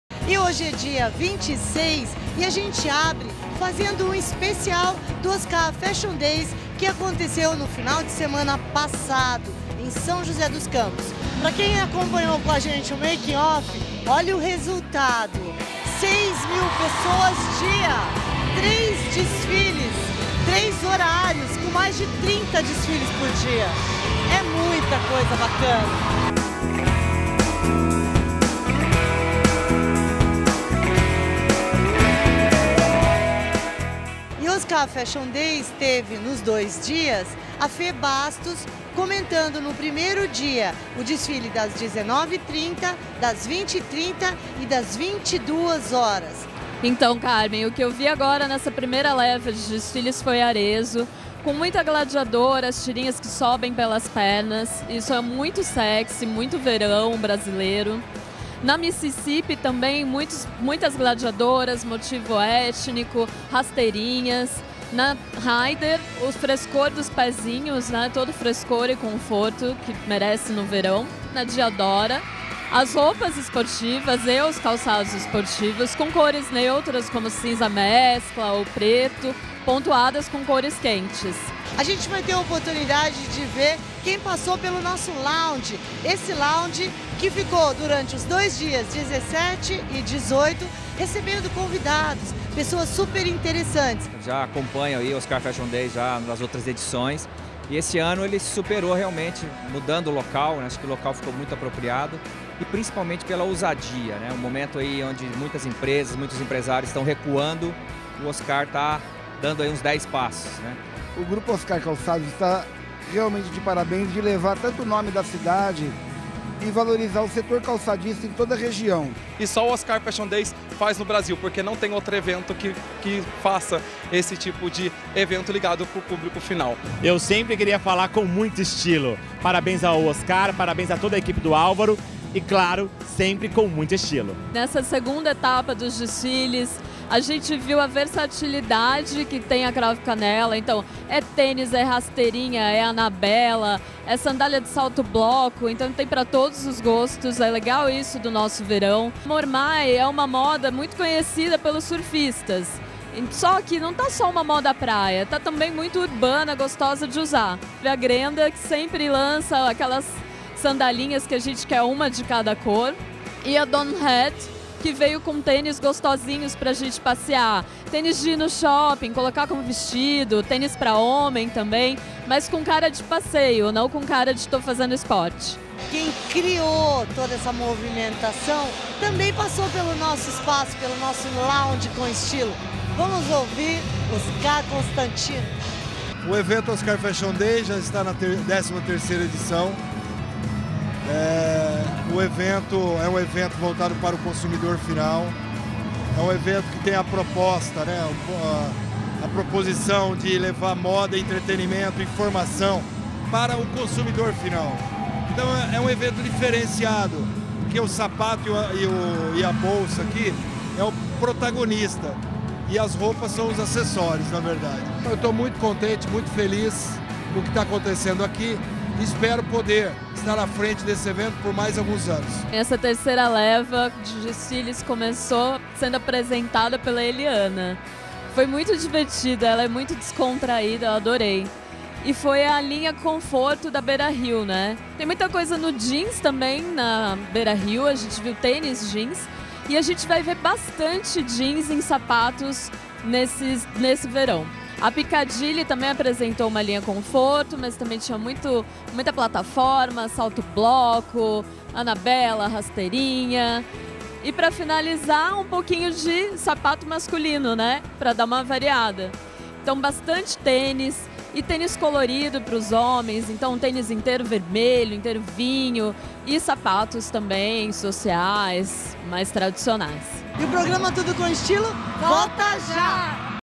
Oscar Fashion Days - Parte 1
Confiram como foi o  maior evento de moda da Região. A Consultora de Imagem Fê Bastos, comentou sobre os desfiles e tendências da primavera- verão, e vejam o que os convidados que passarem pelo nosso launge Carlos Santis (FAAP), Willian Roggles (WR Modelos) Alex Cursino (Consulto de Moda) disseram. E também assista o que o Oscar Constantino disse sobre o seu evento